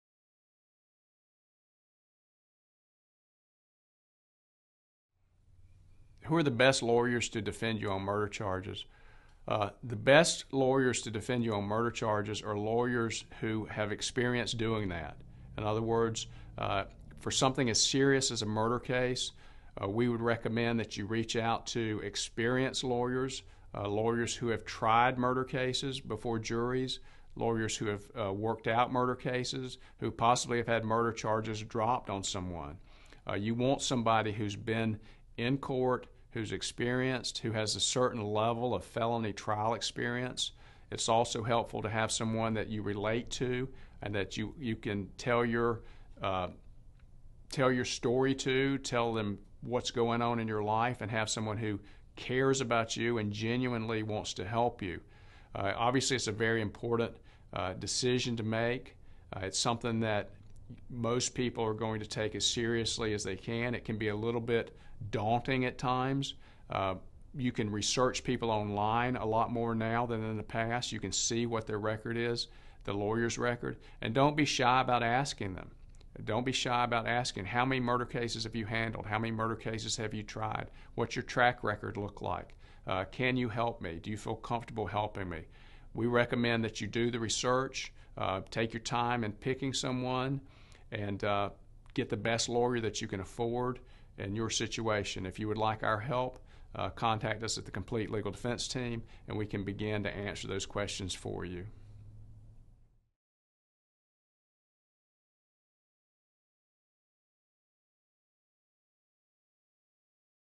Who are the best lawyers to defend me on murder charges?
The best lawyers to defend you on murder charges are lawyers who have experience doing that. In other words, for something as serious as a murder case, we would recommend that you reach out to experienced lawyers; lawyers who have tried murder cases before juries; lawyers who have worked out murder cases, who possibly have had murder charges dropped on someone. You want somebody who has been in court, who is experienced, who has a certain level of felony trial experience. It’s also helpful to have someone that you relate to and who you can tell you story to, tell them what’s going on in your life, and have someone who cares about you and genuinely wants to help you.

Obviously, it’s a very important decision to make. It’s something which most people are going to take as seriously as they can. It can be a little bit daunting at times – you can research people online a lot more now than in the past, you can see what their record is, and don’t be shy about asking them! Don’t be shy about asking: how many murder cases have you handled? How many murder cases have you tried? What’s your track record look like? Can you help me? Do you feel comfortable helping me?

We recommend that you do the research, take your time in picking someone, and get the best lawyer that you can afford for your situation.

1012 38th Ave. N.
Myrtle Beach, SC
United States